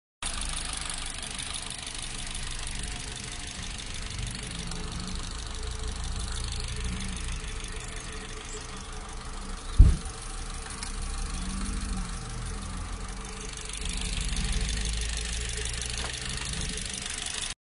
Cimitero monumentale caltanissetta
Lo stato in cui verte il cimitero monumentale di caltanissetta